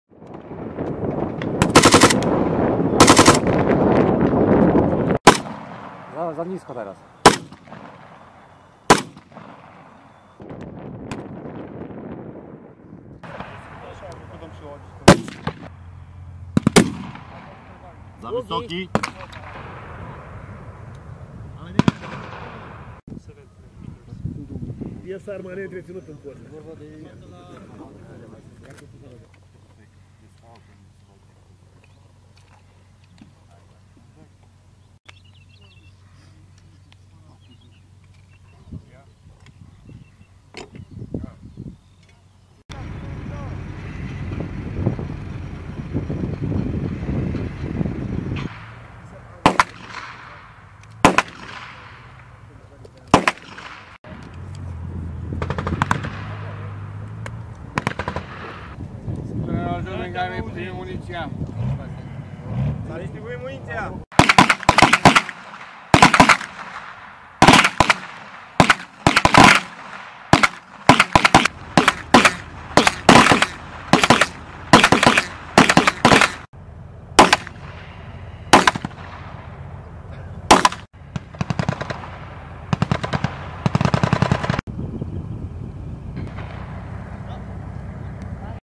Multinational Range NATO Battle Group Poland
Battle Group Poland soldiers conduct multinational weapons marksmanship at Bemowo Piskie Training Area, Poland, on May 8, 2018.
Video by Spc. Hubert Delany/22nd MPAD